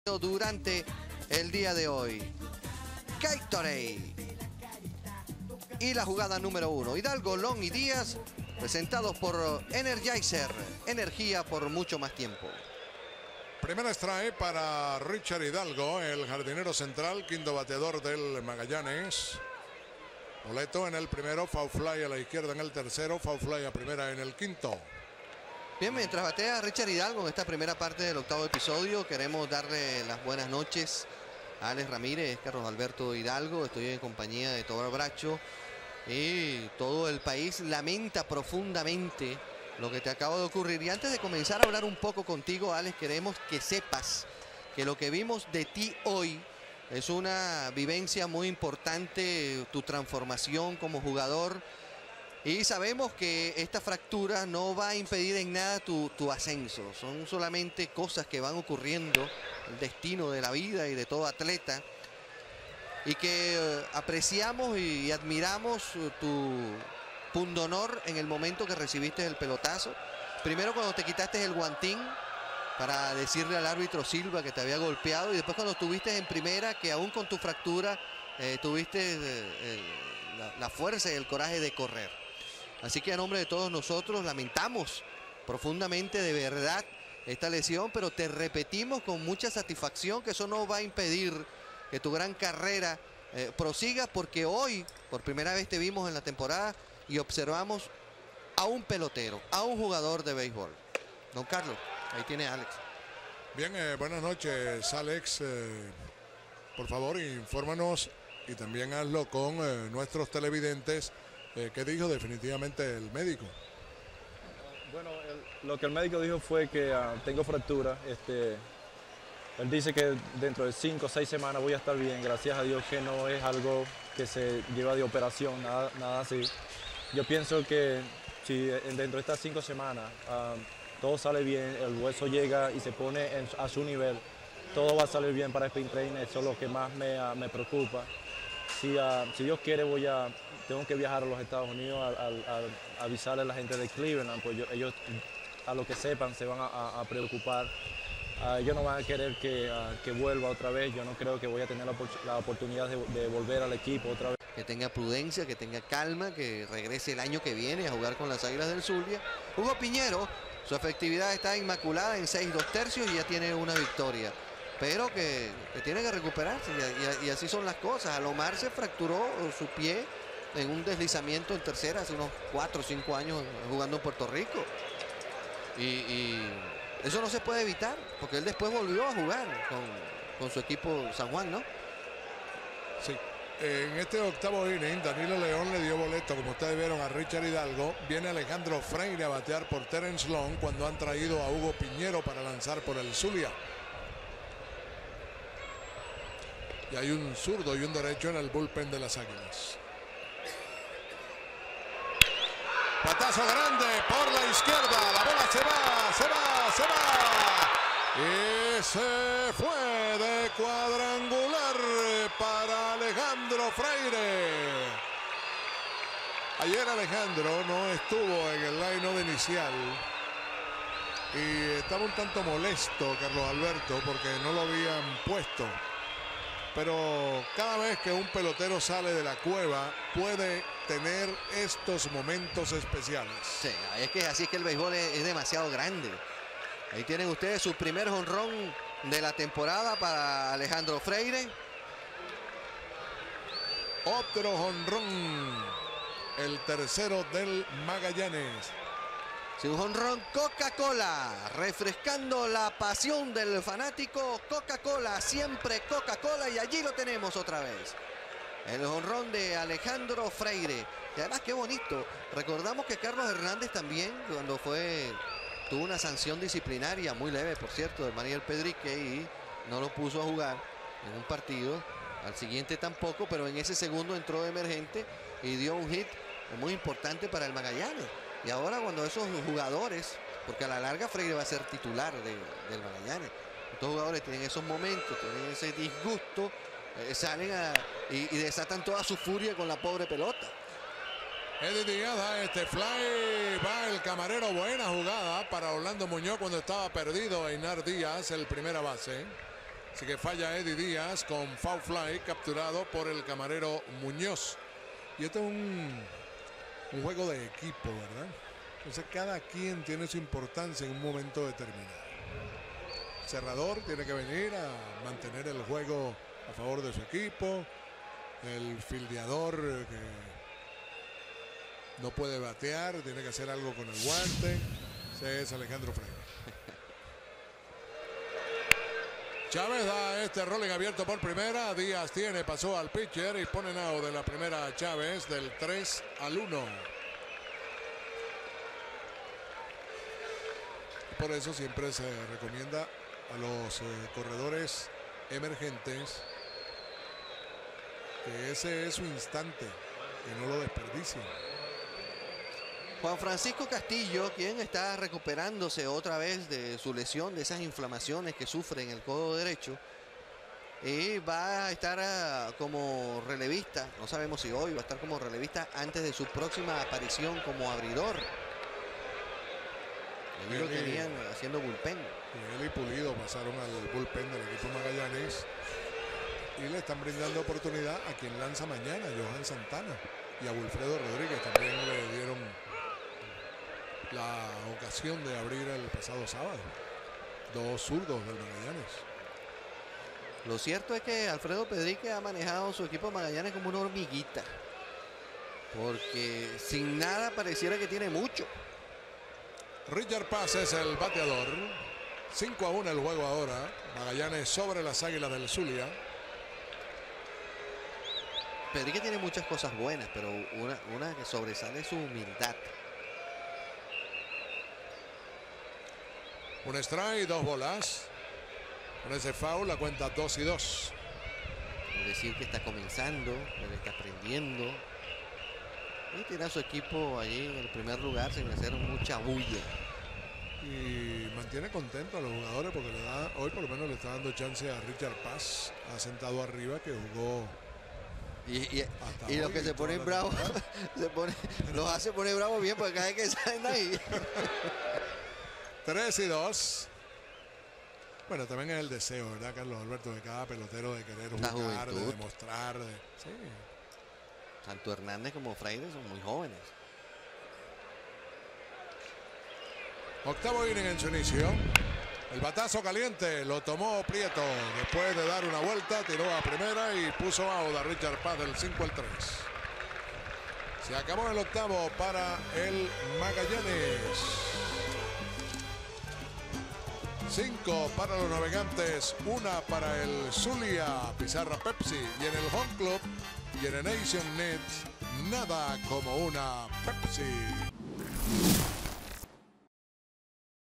Jonron de ALEJANDRO FREIRE LVBP Temporada 1998-1999 Águilas del Zulia vs Navegantes del Magallanes
Cuadrangular de Alejandro Freire el 17 de Noviembre de 1998 en el encuentro entre los Navegantes del Magallanes y las Águilas del Zulia en el estadio Luis Aparicio "El Grande" de Maracaibo, temporada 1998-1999 de la Liga Venezolana de Beisbol Profesional.

Cortesía Venevision